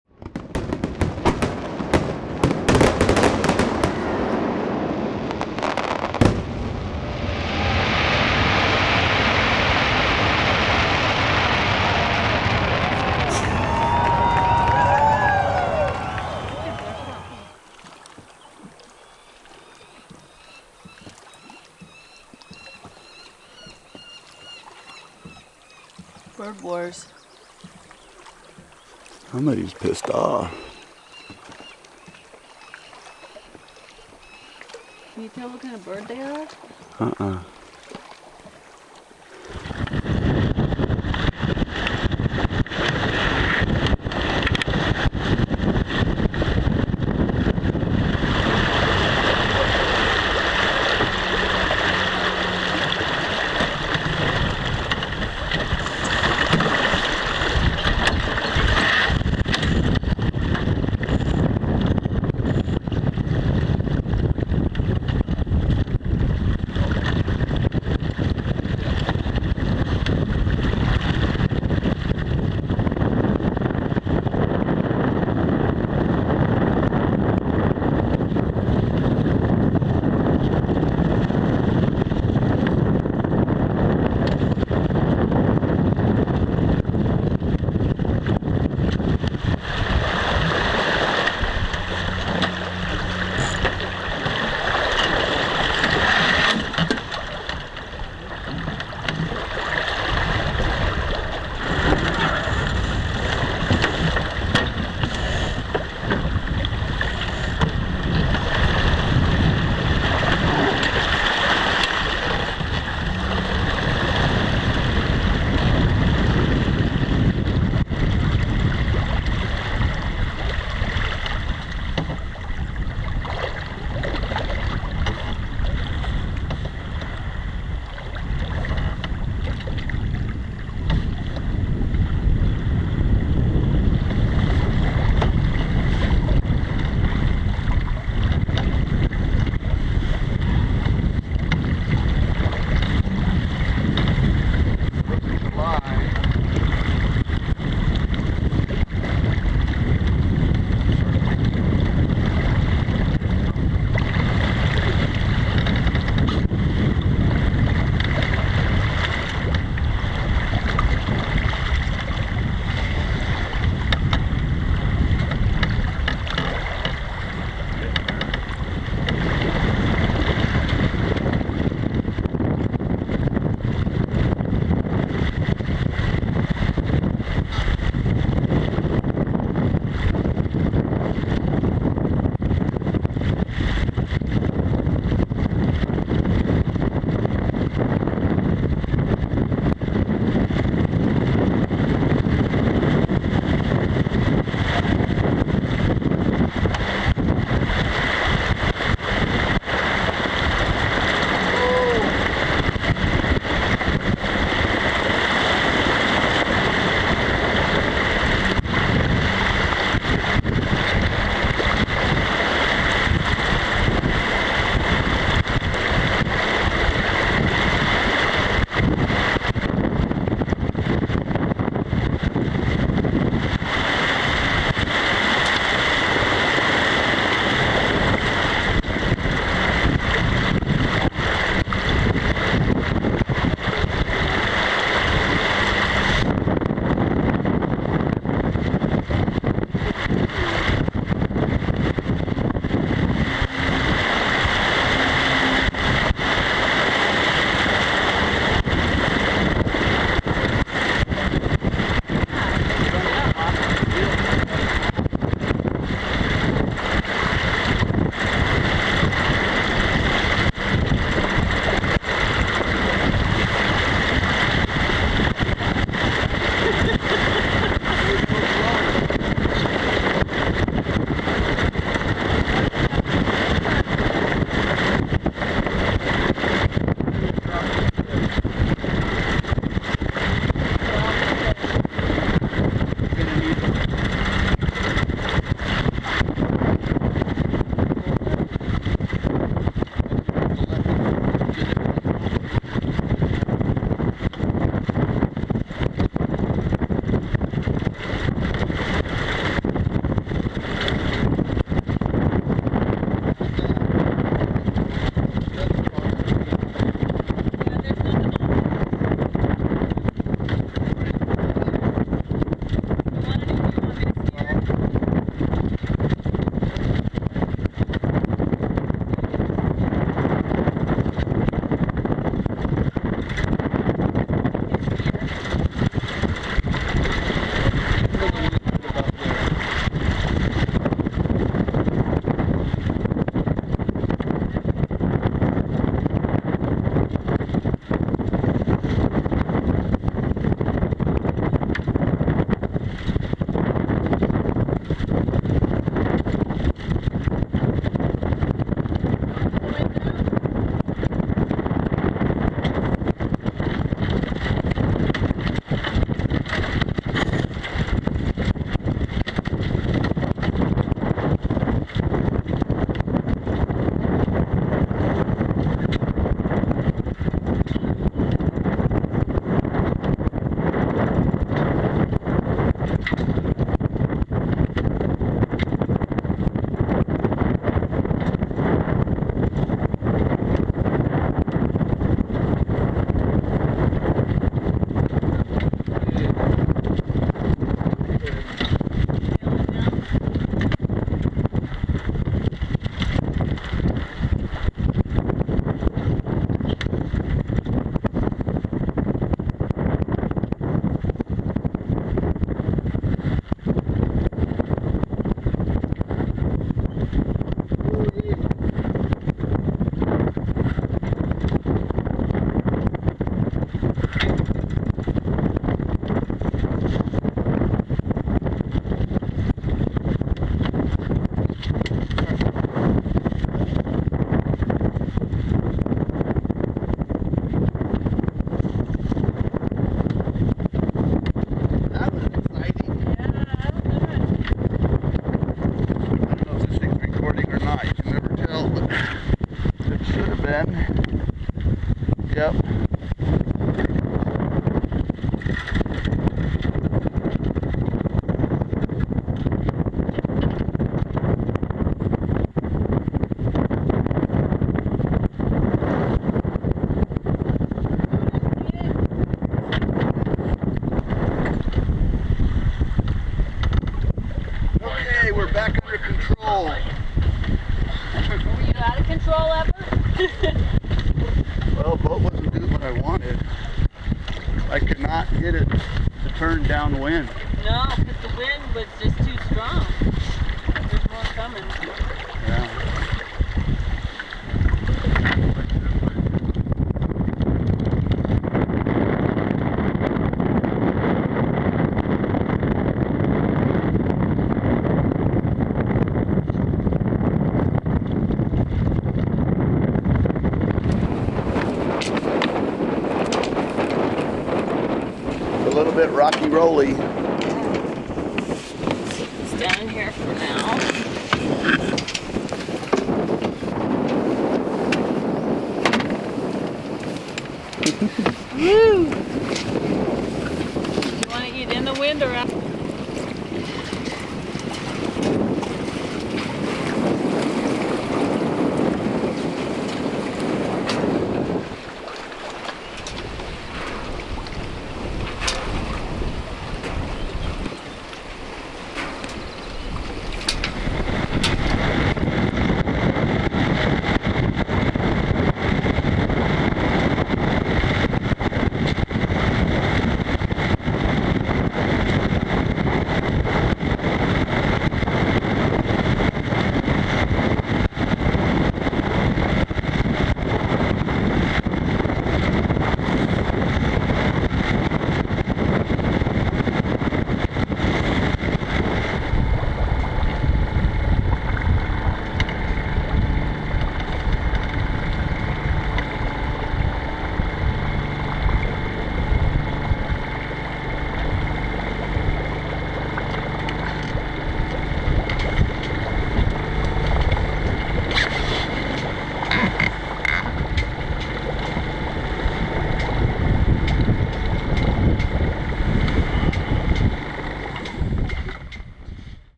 PB74510.mpg
2010 July 4th weekend, the 4th starts out nice, and winds build. Nice downwind run with 15-20 mph wind, we hit over 7mph and had steady cruisin at 6... then a gusty jump to 40+ that stays with us for a nice visit